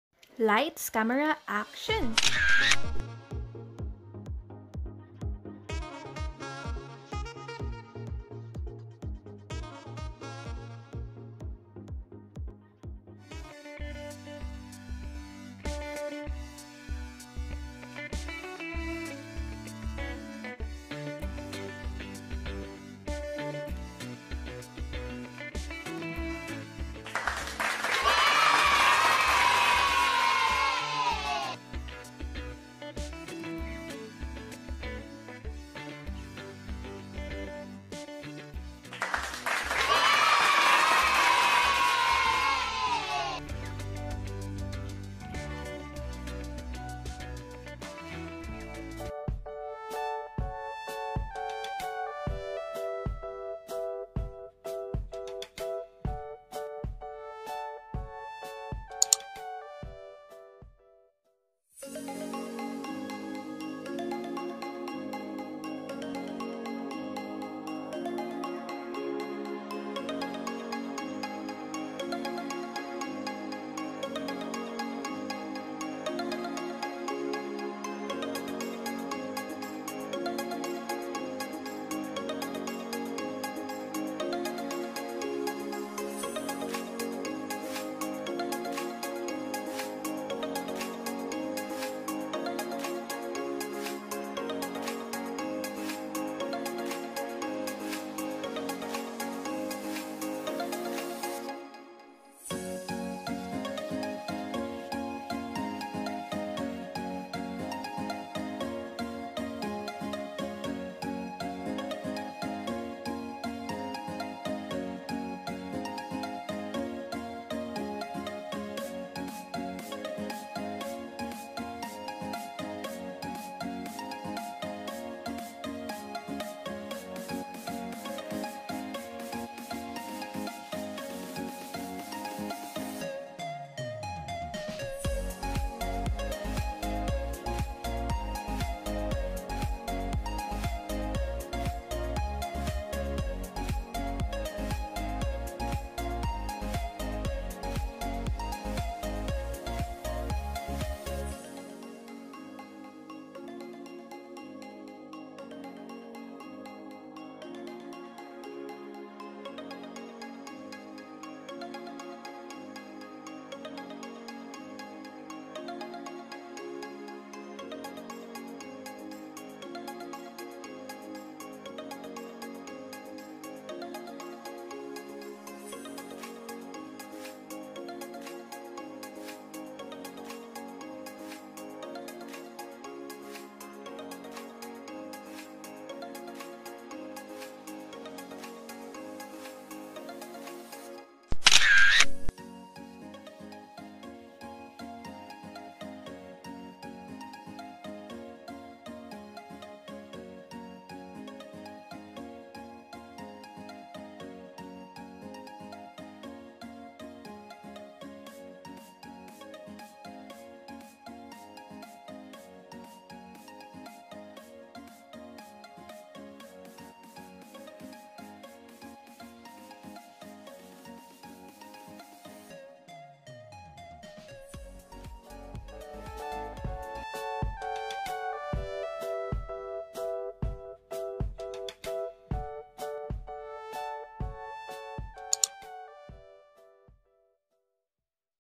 CRISPY MILO STICKS | Tiyak na swak sa bata man o matanda | Namae's Kitchen and Tutorials
Hope you'll like this recipe 😍😋

Today, I will share to you my crispy milo sticks. I tried this to make a different kind of merienda especially to my niece and nephews. They really love it and asked for more.